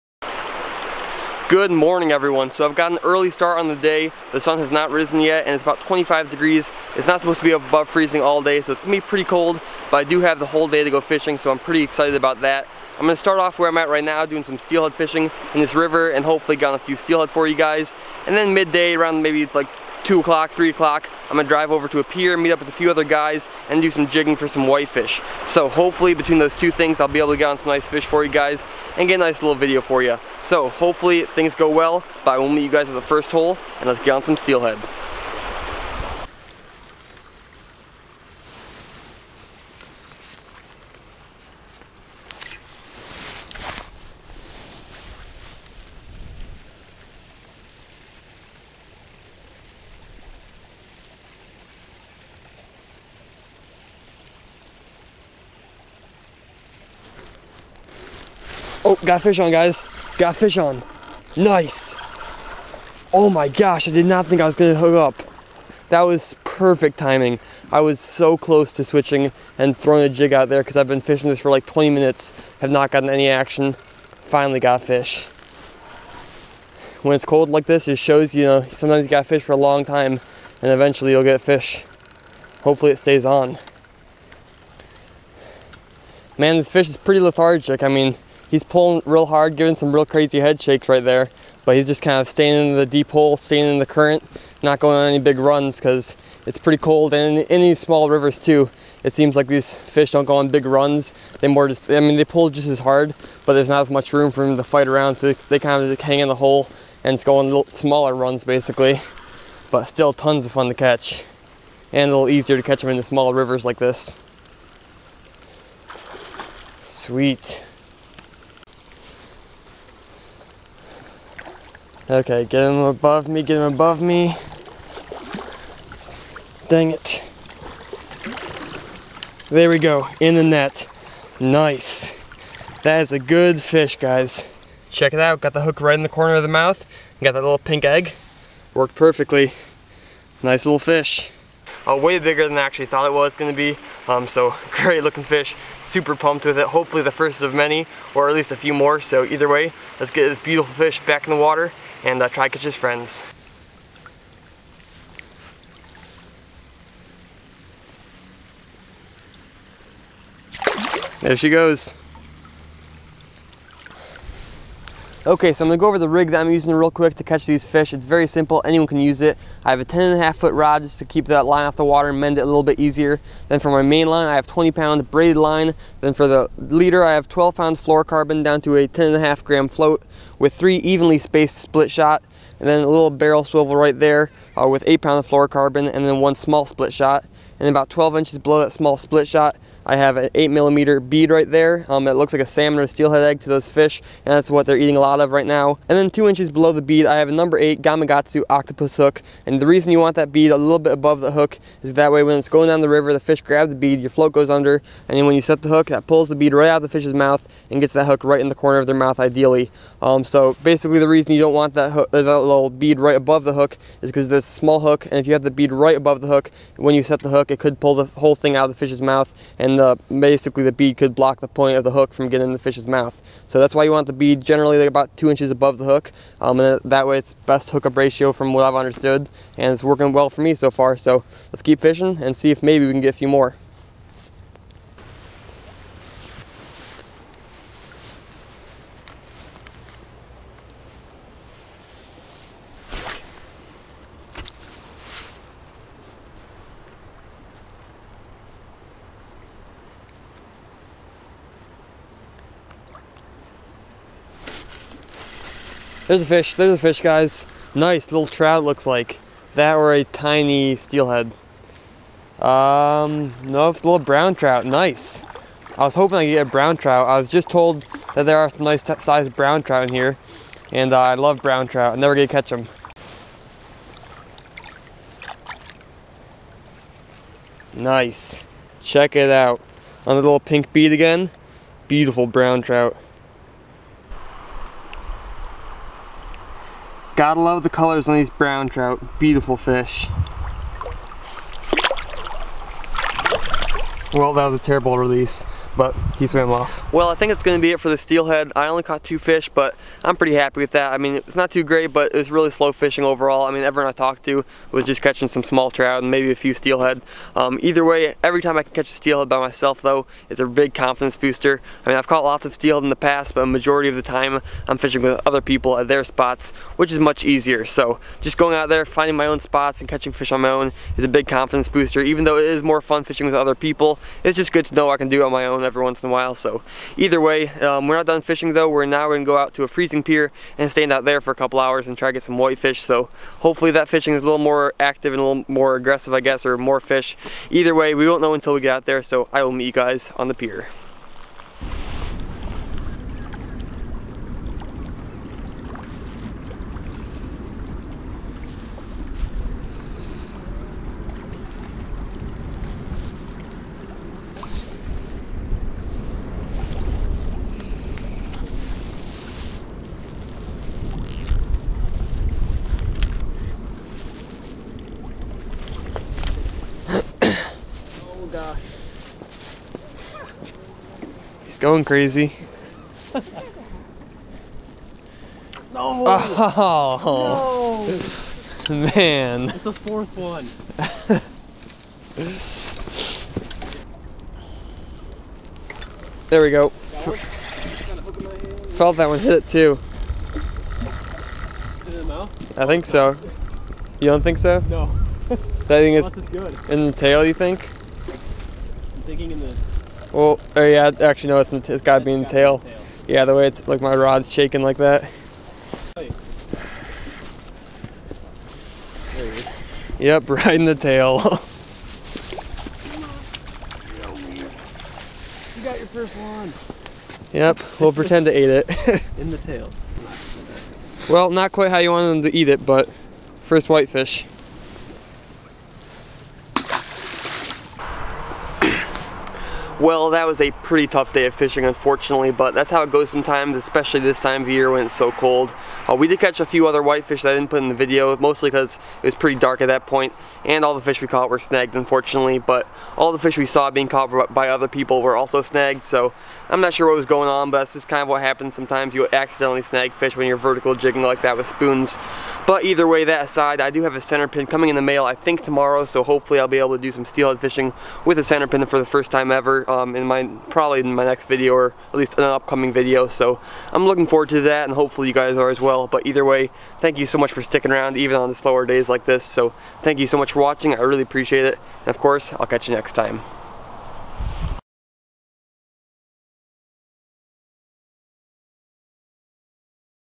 Steelhead Fishing the Easiest Rig Ever
Setting out on a cold winter day in search of some hungry steelhead. Despite the cold conditions, using a simple rig I was able to land some nice fish!

Add me on Snapchat/Instagram @peelindrag

Chest cam: Gopro Hero 3+
DSLR: Canon T6I